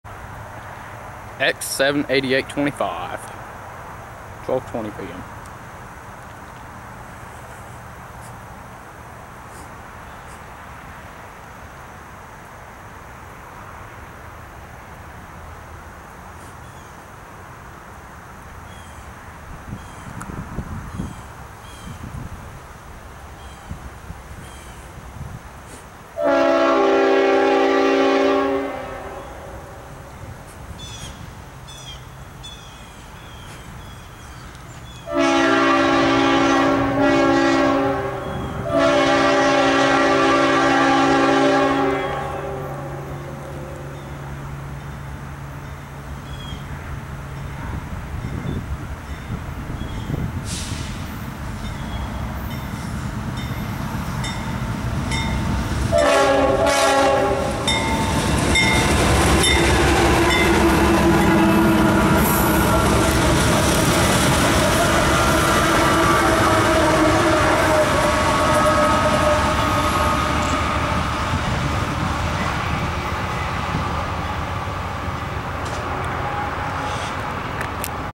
HD: CSX 7703 leads CSX X788-25 in Moreland, GA
S.E. Moreland
9/26/2015
12:21p.m.
CSX: X788-25
CSX: 7703(CW40-8)
CSX: 143(CW44AC)
CSX: 7887(CW40-8)
CP: 8806(ES44AC)
Southbound
Power Move